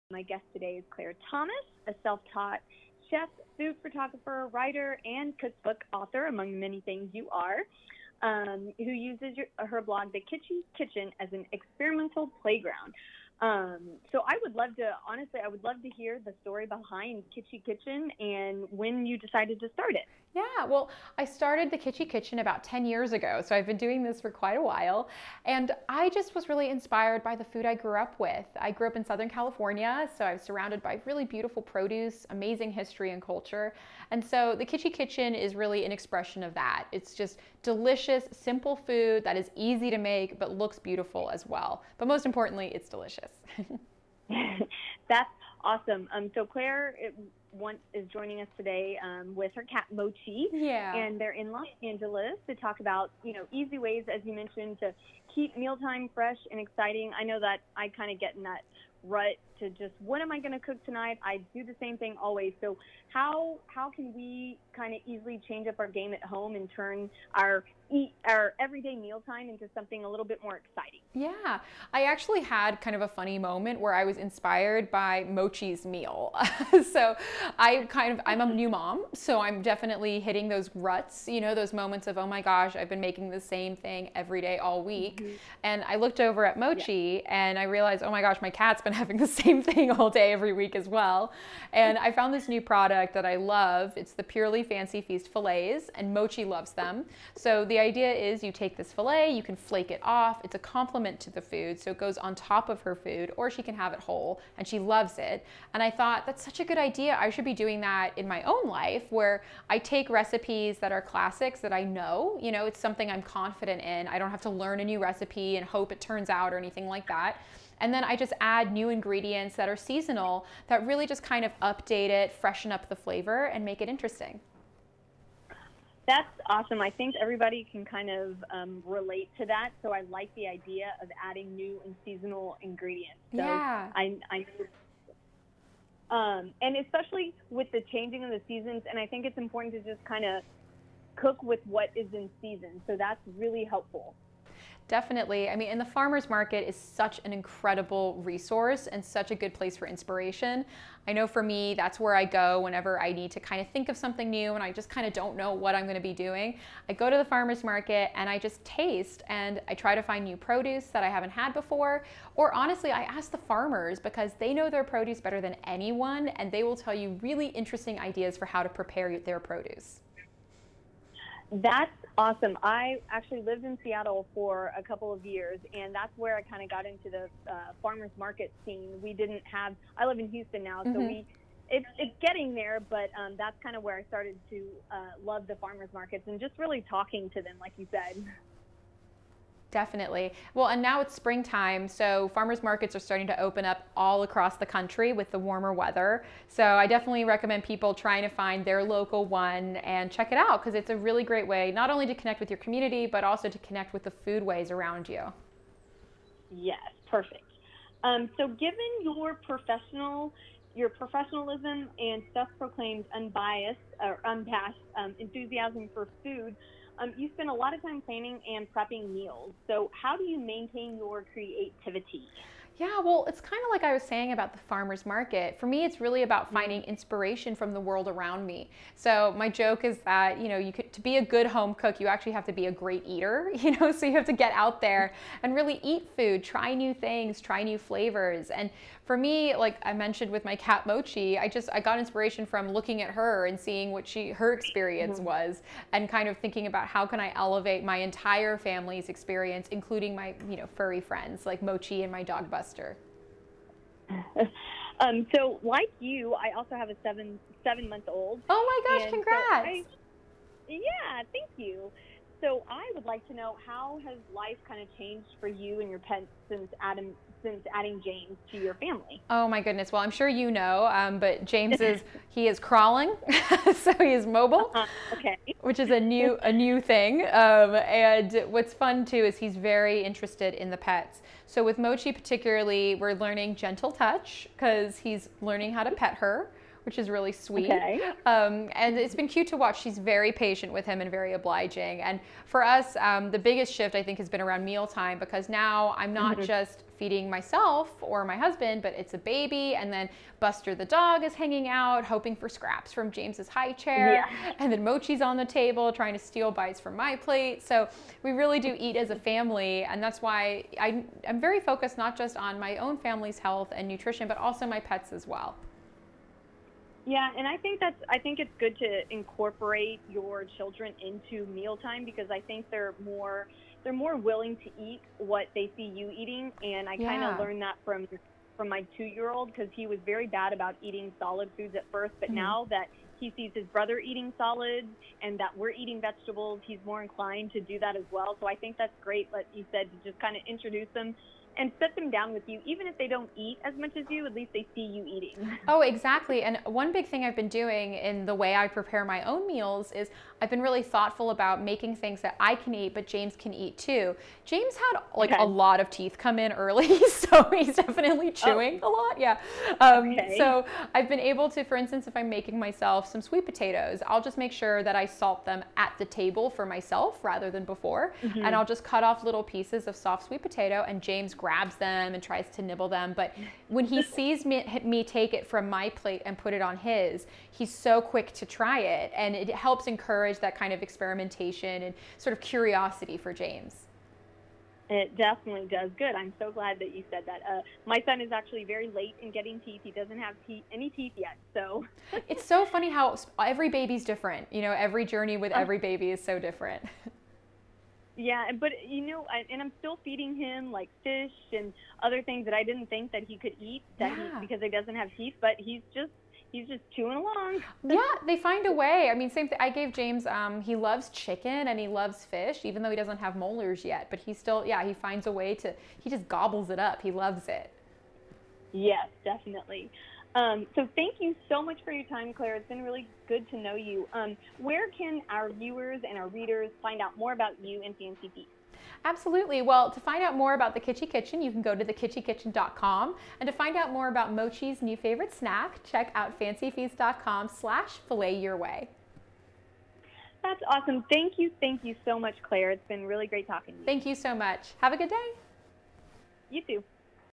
Claire Thomas of Kitchy Kitchen Blog Talks All Things Culinary to Sabrina Skiles
Claire Thomas of the Kitchy Kitchen Blog Talks All Things Culinary to Sabrina Skiles of Homegrown Houston. We discuss tips for staying inspired at mealtime, how mealtime has changed for her since Claire has become a mother and much more.